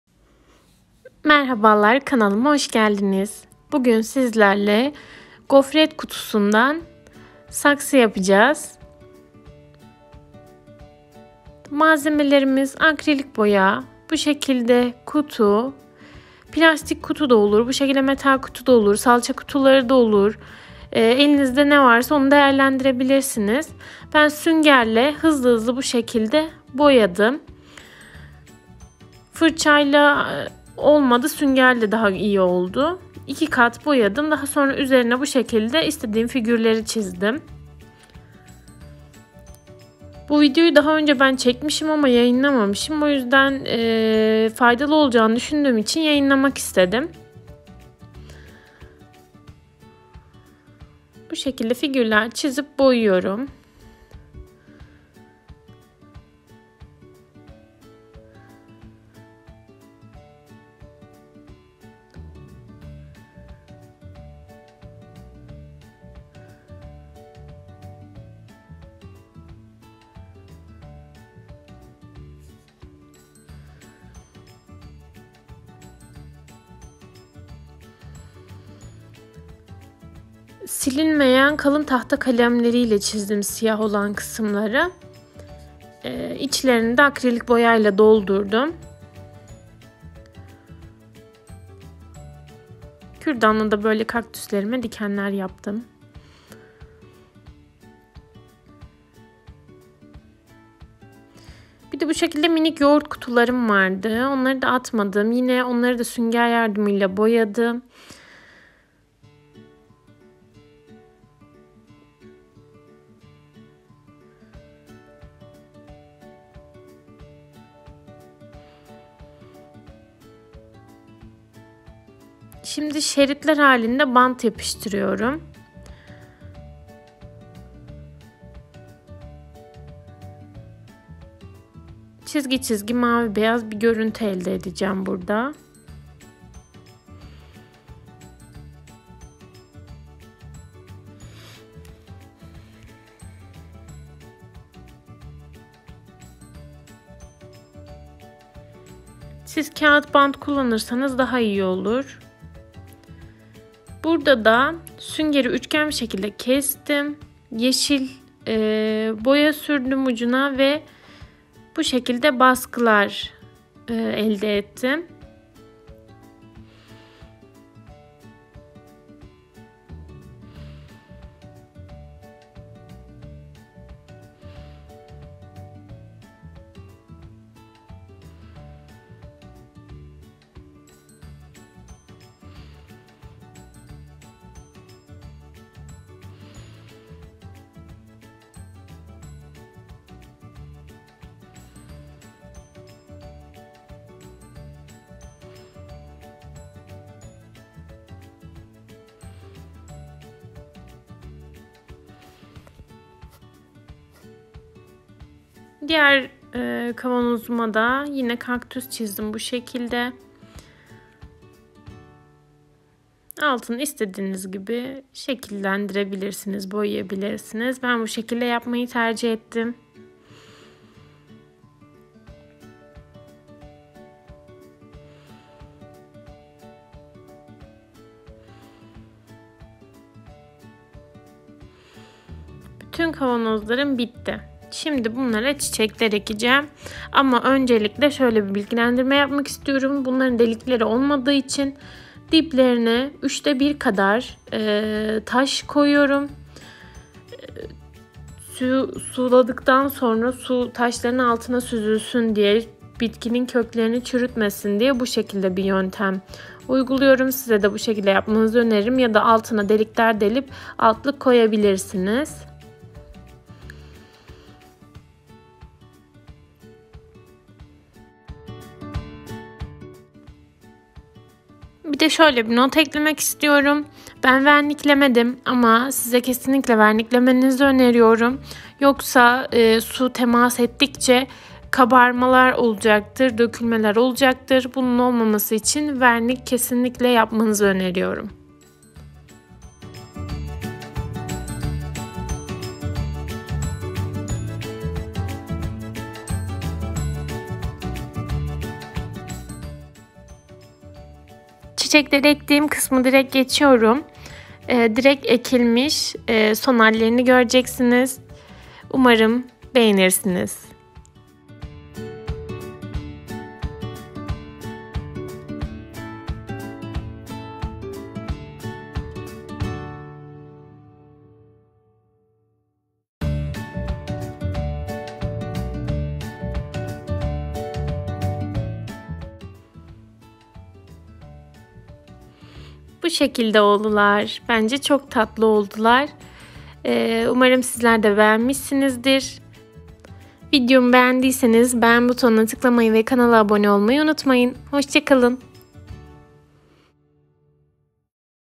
Gofret Kutusundan Saksı Yapımı | Kendin Yap | DIY | flowerpot painting
Merhabalar :)

Bu gün atmak yerine değerlendirmeyi tercih ettiğim gofret kutularının saksıya dönüşümünü gerçekleştirdik. Bence çok tatlı oldular ben çok beğendim ☺️ Sizlerde Videomu beğendiyseniz beğen butonuna tıklamayı unutmayın ❤️

Diğer videolarda görüşmek üzere hoşçakalın 😊👋🏻

Önceki Videolarım 😊